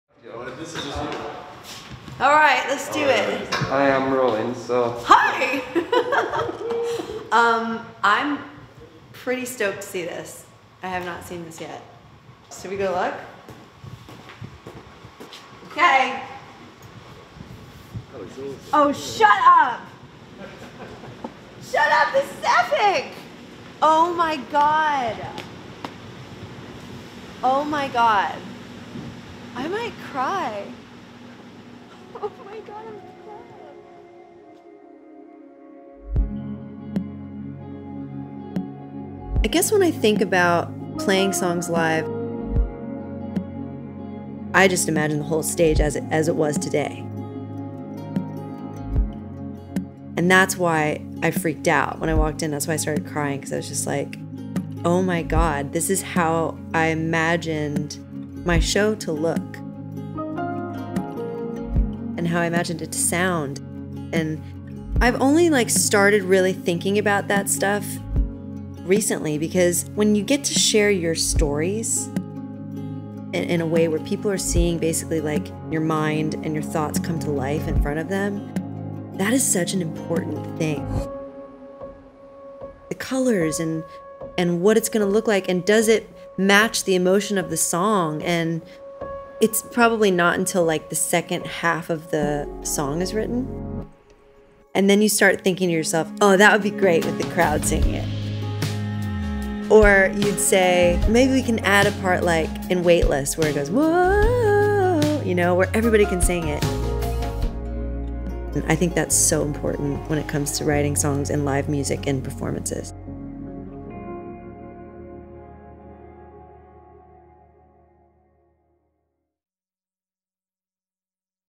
What live music means to me - Alyssa Bonagura
In #2020 I was able to film 12 brand new songs with the incredible team at AdLib in Liverpool, England. So excited to share each one with you this year!!
xx
Alyssa